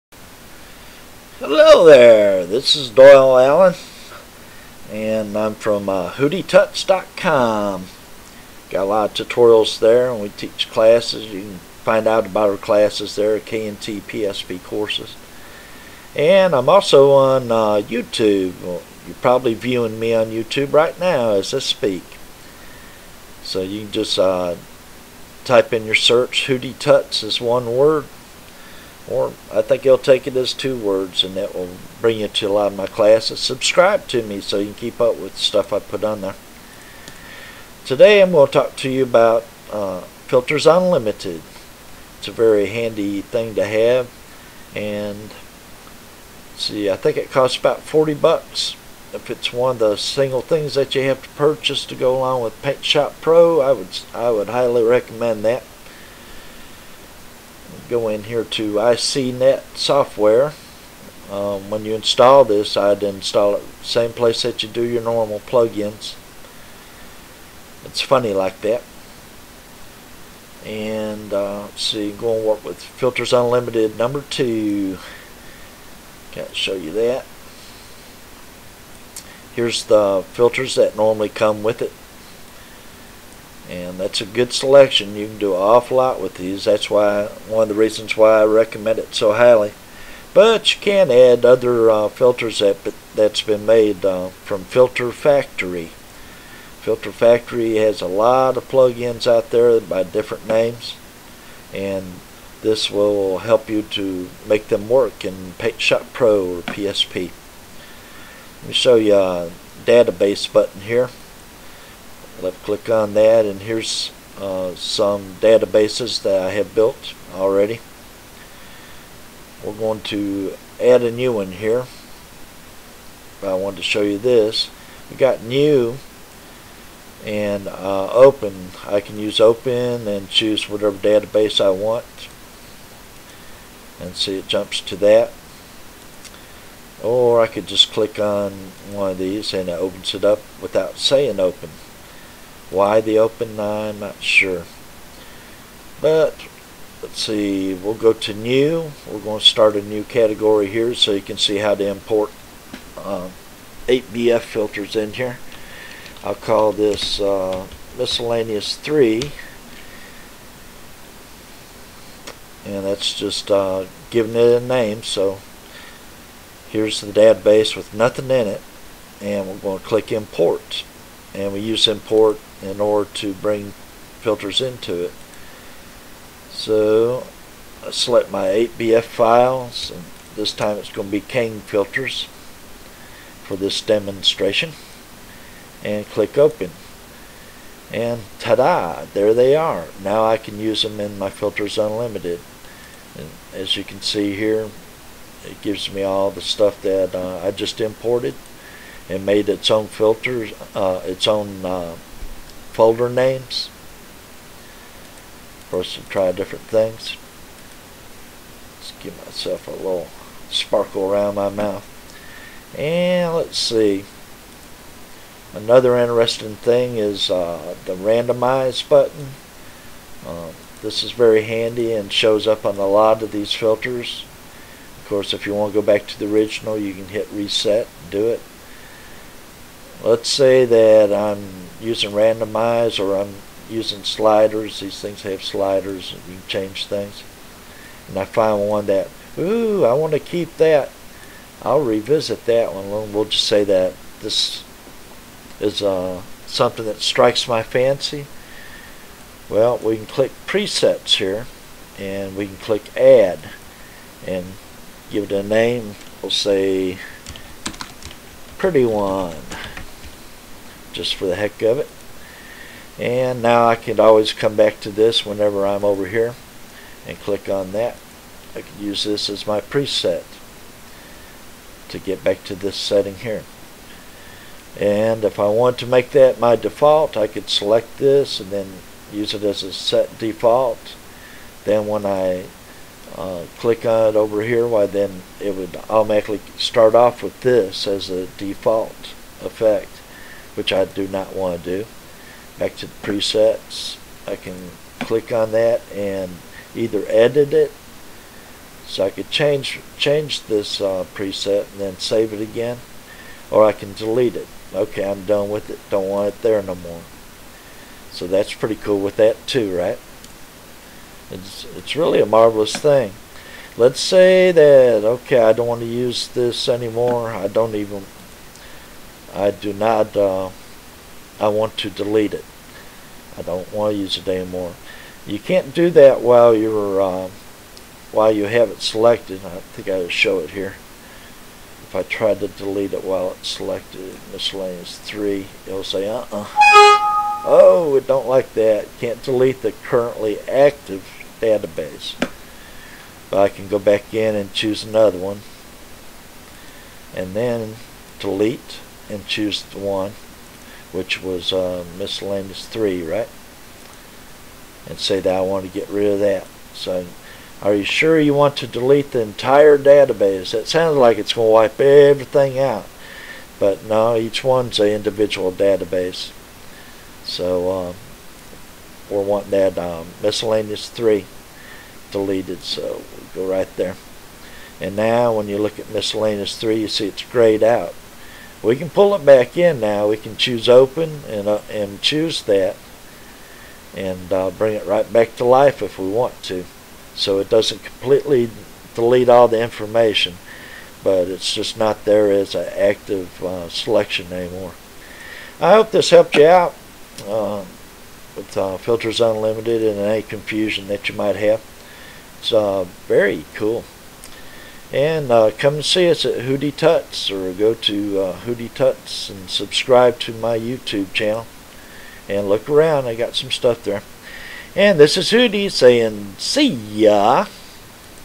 ICNets Filter Unlimited Explained - HootyTuts
This HootyTut by Doyle Allen shows you some insights in using ICNets Filters
Unlimited program. How to add Filter Factory plugins, random, edit, delete,
open, new, make default buttons all explained. Visit and join my Paintshop Pro